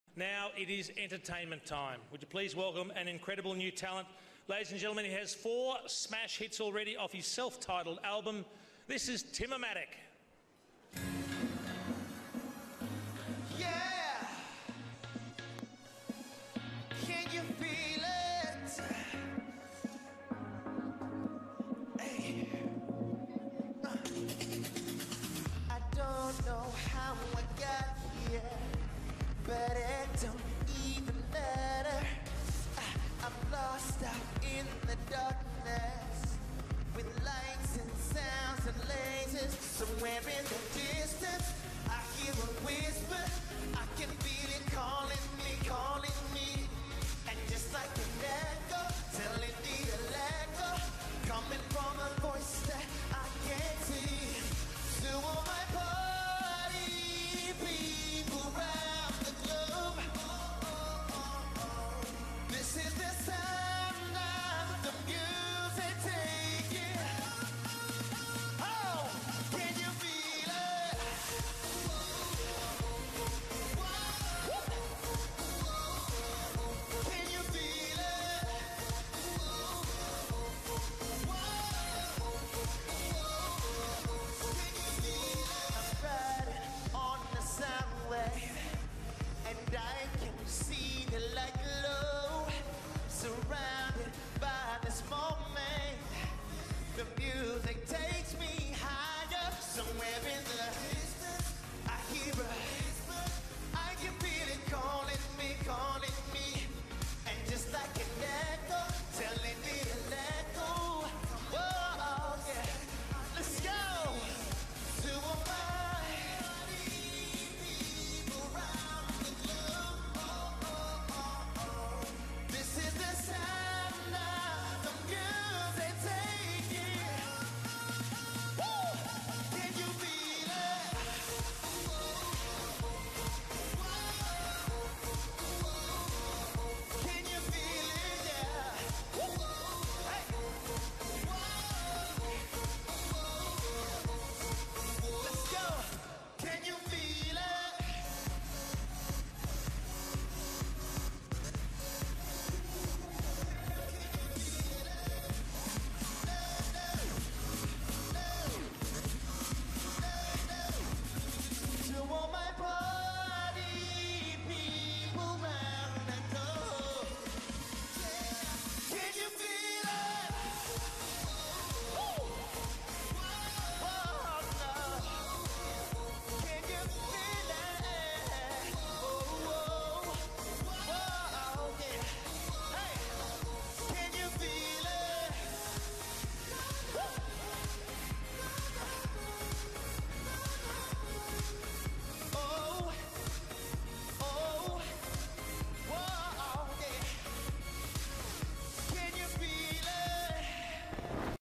TIMOMATIC LIVE AFL MVP sept 2012 FULL HD
Timomatic sings from his smash hit album on fox footy MVP awards night